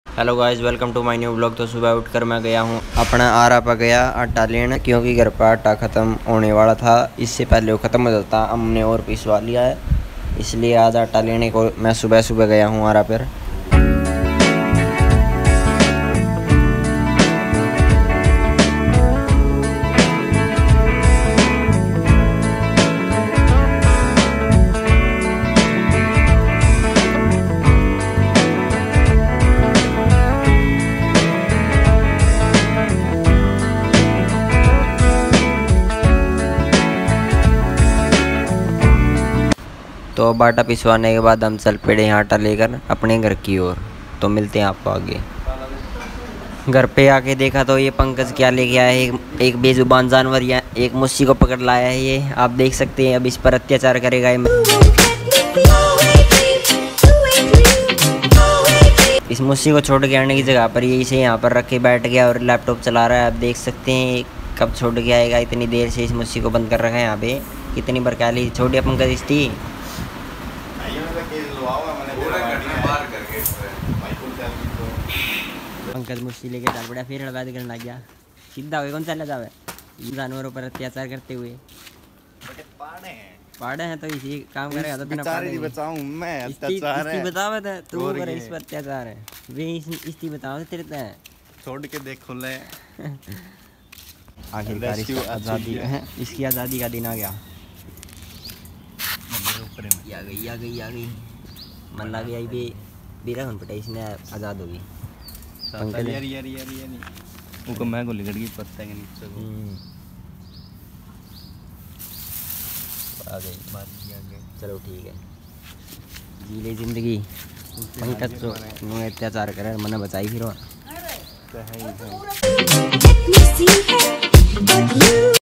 Chuhe ke sath kya huaa🫣
Pinjera me fas gya chuha😰

This is varun jangra

Desi vlogger